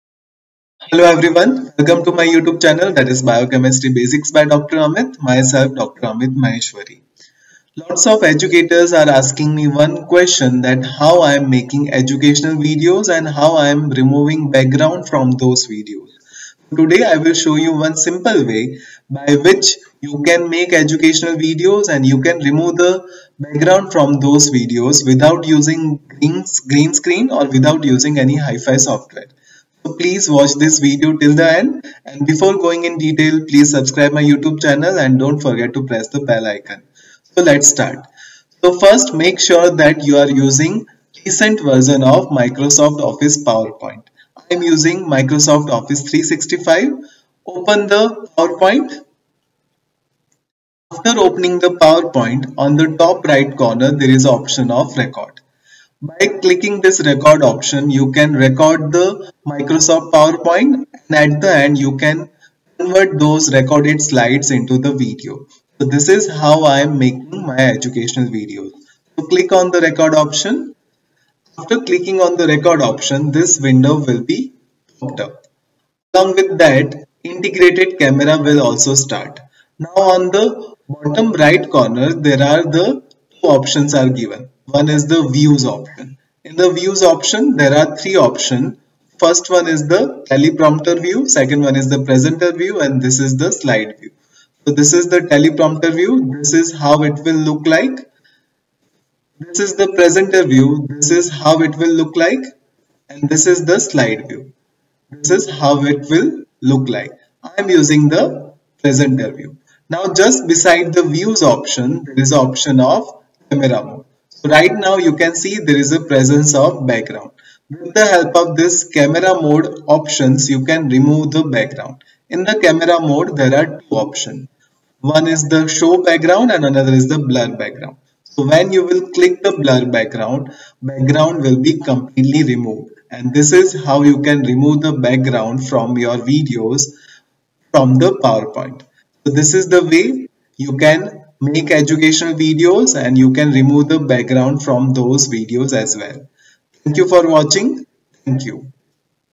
How To Make Videos And Remove The Background From Videos Using PowerPoint In Two Minutes
This is the short video on how to make videos using PowerPoint and how to remove the background from videos using PowerPoint without use of green screen which will be helpful to educators.